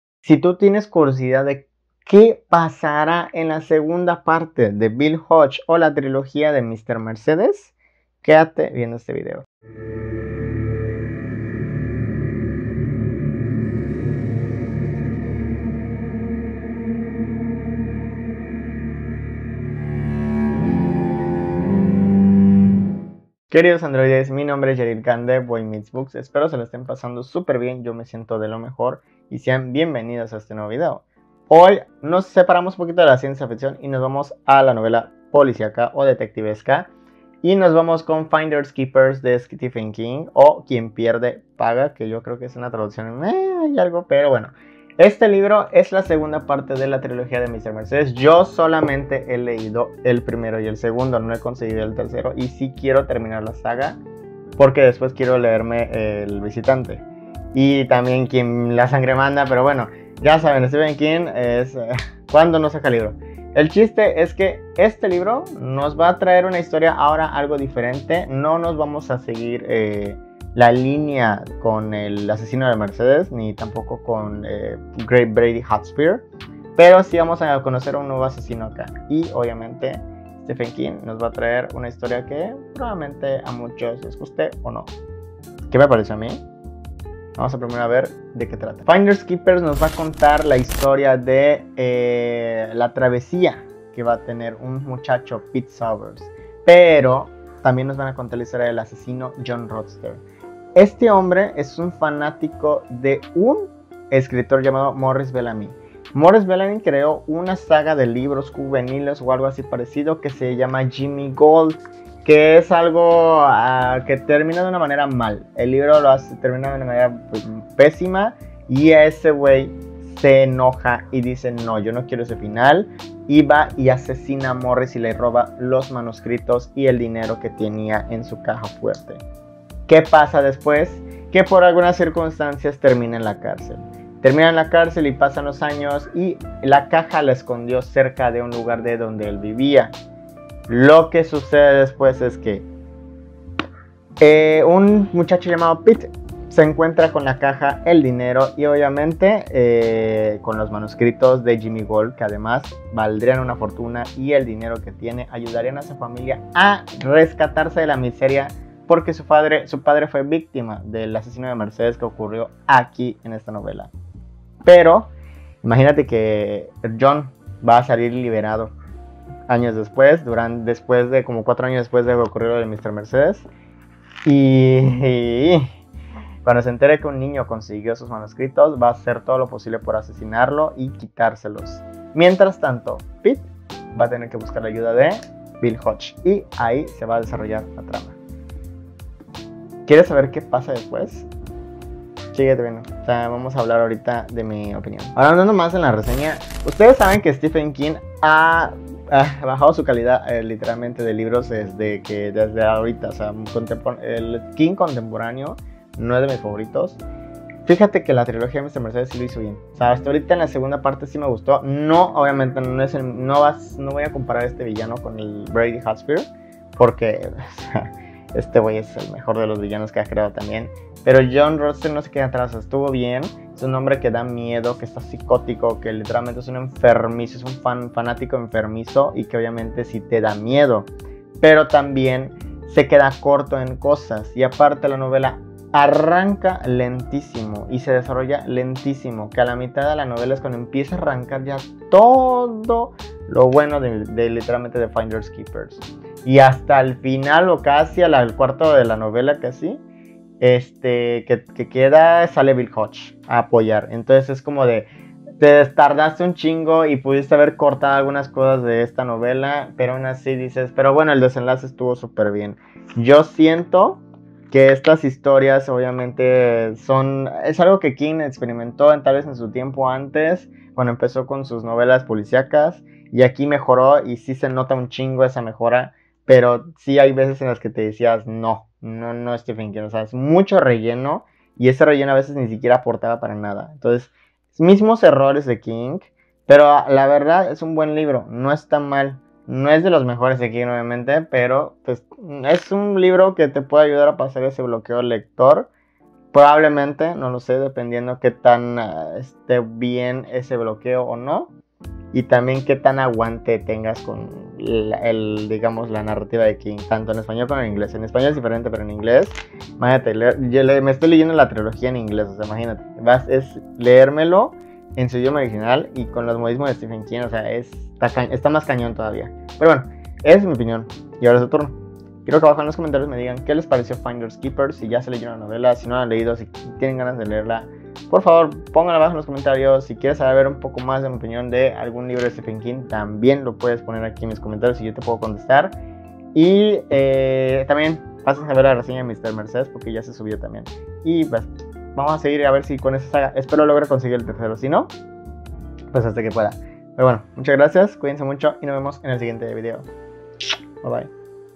💀QUIEN PIERDE PAGA de Stephen King💀 | Boy Meets Books Mx
¡Hola mis androides! Este video va a tratarse de una de las novelas más icónicas del escritor Stephen King.